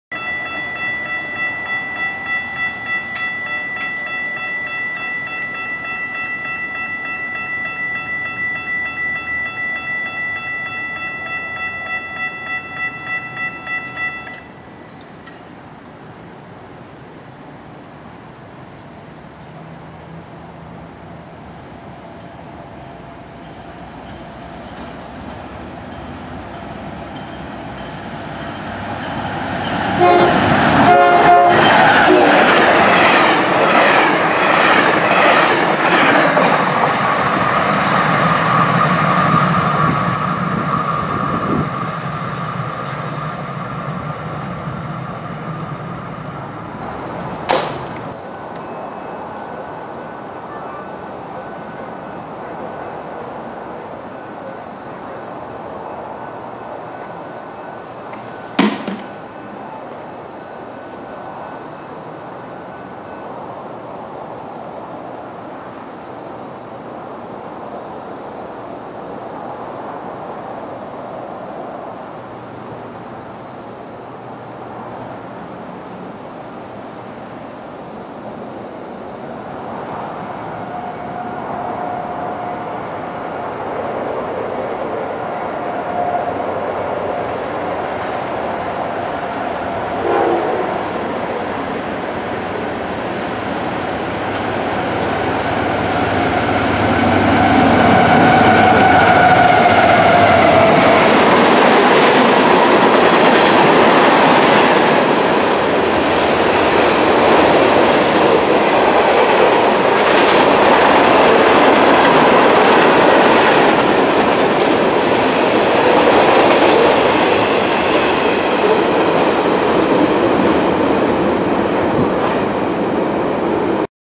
[HD]Fast Moving Trains After Hours on the RTA 6/21/2020
1. Tri-RailP981-21 Southbound Passenger Train

2. CSXTQ453-21 Waycross, GA - Miami, FL Pulling 22 Cars.

    Csx#418(AC44CW)
    Csx#8904(EMD ST70AH)